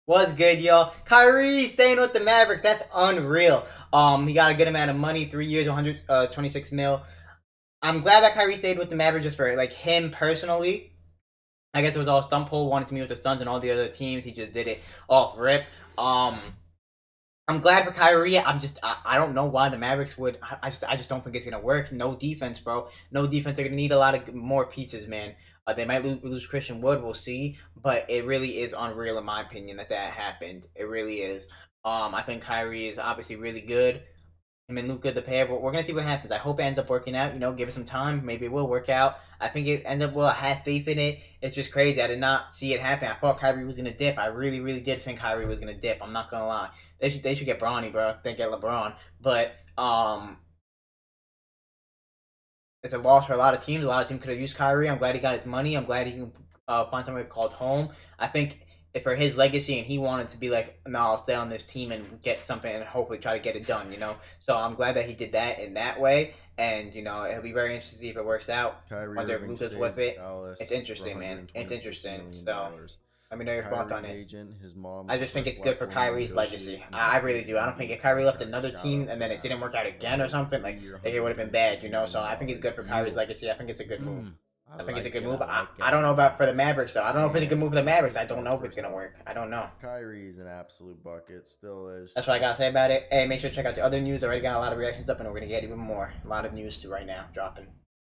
SHOCKED! KYRIE IRVING STAYS WITH MAVERICKS 2023 REACTION!
IN THIS VIDEO I REACTED TO KYRIE! LET ME KNOW YOUR THOUGHTS